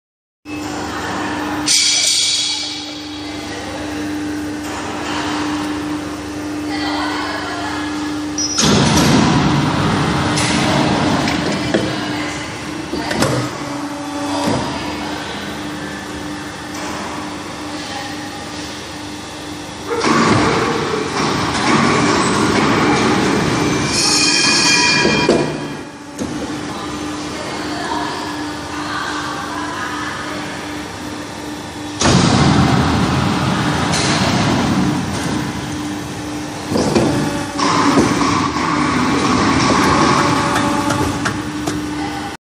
Telhoo SG-II-80 end forming machine for Rectangle tube reducing
This machine can make tube end reducer, expander, flange and flare depend on the different molds. To know more information, please contact with us directly,